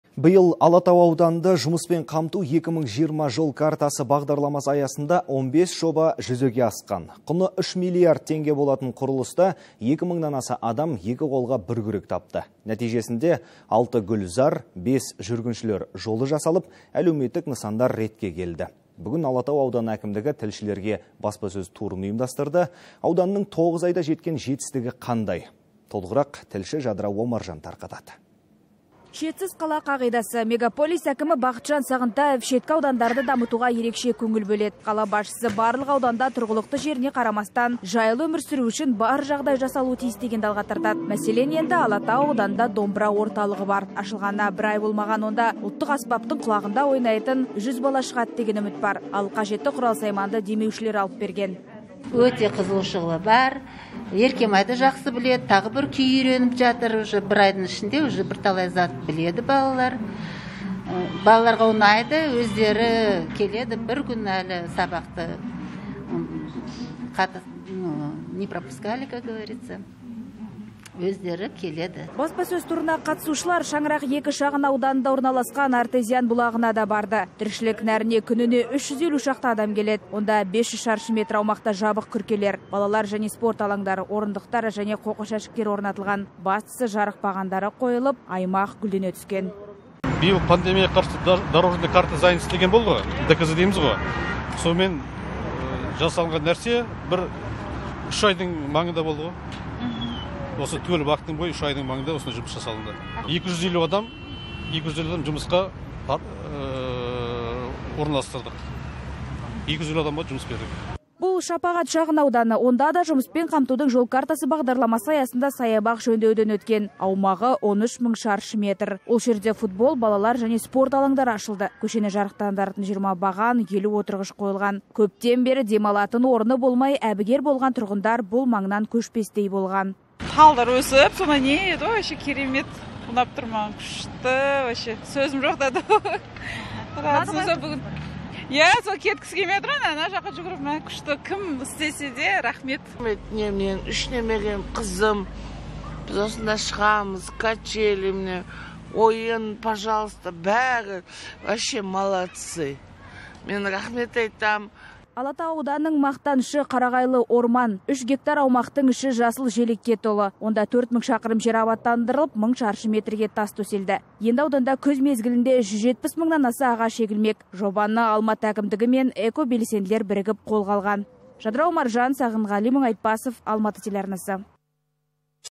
Шетсіз қала: Алматының ең жас ауданы қалай өзгеруде
22 қазанда Алматы қаласы өңірлік коммуникациялар қызметінің ұйымдастыруымен Алатау ауданында аудан әкімі Азамат Қалдыбековтың қатысуымен баспасөз туры және баспасөз конференциясы өтті, деп хабарлайды Almaty.tv.